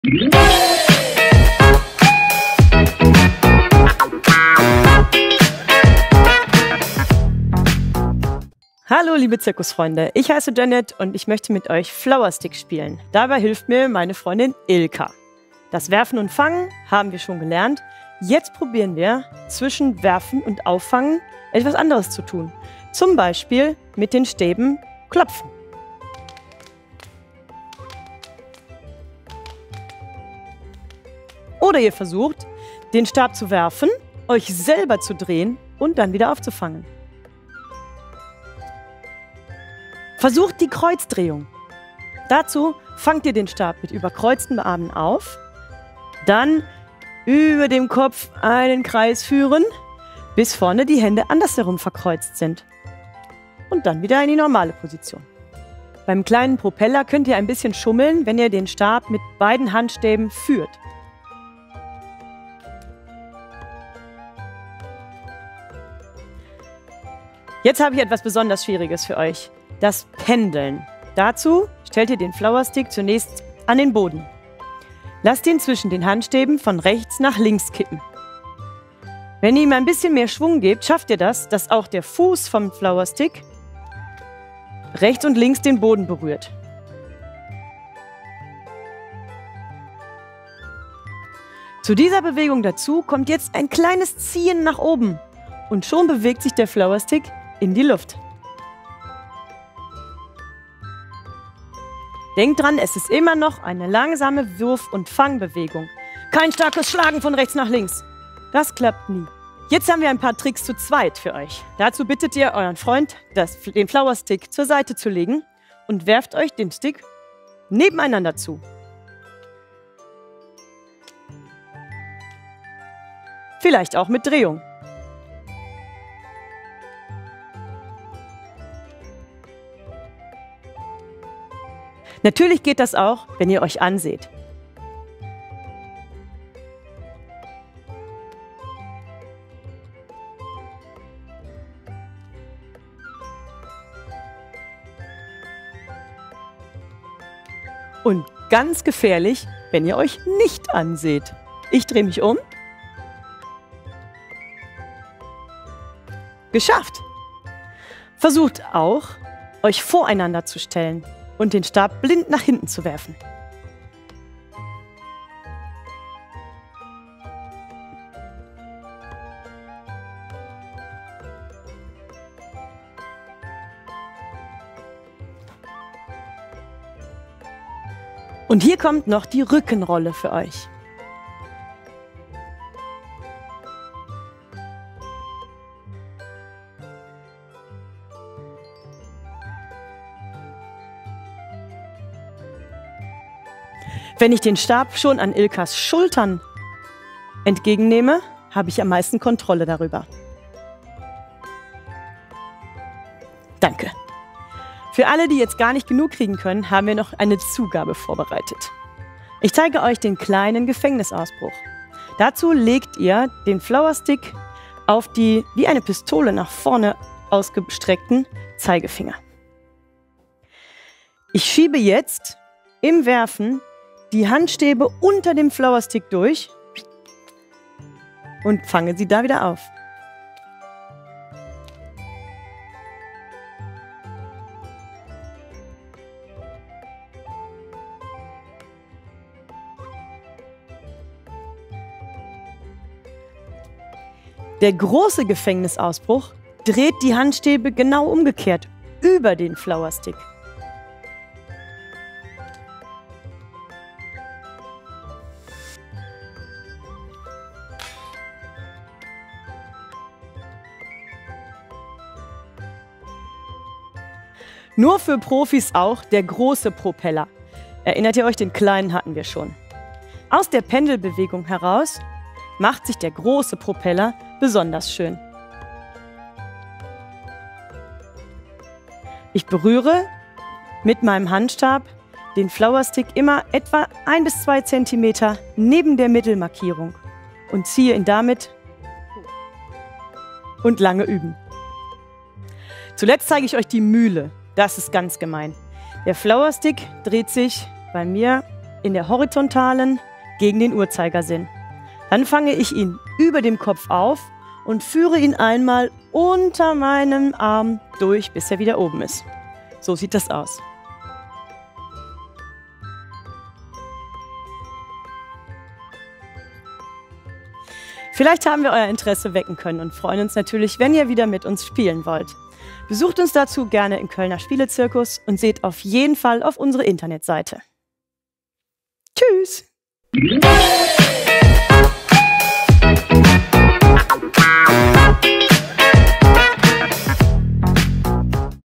Tutorial: Flowerstick spielen – weitere Tricks (2)
Im Rahmen unserer Zirkusprojekte an Schulen mit dem Kölner Spielecircus haben wir zahlreiche Video-Tutorials von unterschiedlichen Zirkustechniken produziert, die zusätzlich zu unseren Handouts, eine Unterstützung für die Nachwuchsartist*innen und Zirkusbetreuer*innen darstellen sollen.

In diesem Video werden weitere Tricks mit dem FLOWERSTICK gezeigt:
00:00 Trick 1: Variationen mit Werfen und Fangen
00:39 Trick 2: Kreuzdrehung
00:55 Trick 3: kleiner Propeller „mit Schummeln“
01:05 Trick 4: das Pendeln / Grundschlag
01:48 Trick 5: zu zweit nebeneinander - Zuwerfen
02:11 Trick 6: zu zweit voreinander - Zuwerfen
02:26 Trick 7: zu zweit hintereinander – Zuwerfen
02:57 Trick 8: die Rückenrolle
03:24 Trick 9: kleiner Gefängnisausbruch
04:04 Trick 10: großer Gefängnisausbuch
04:23 Trick 11: großer Propeller
04:51 Trick 12: die Mühle